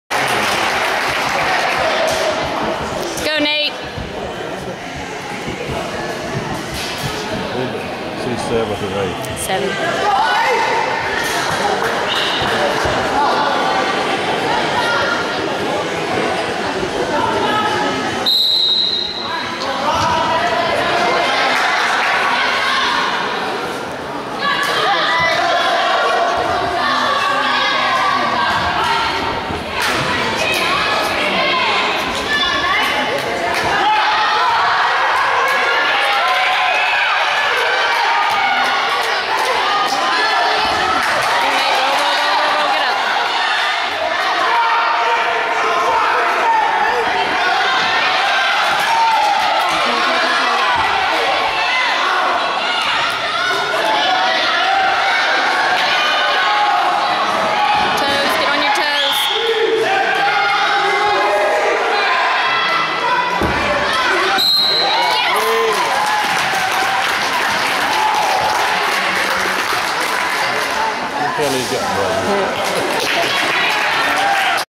Nate's first middle school wrestling match
Crest Middle School vs. Chase. 11/2/2010
Wrestling 103 lbs while weighing in at 93 lbs.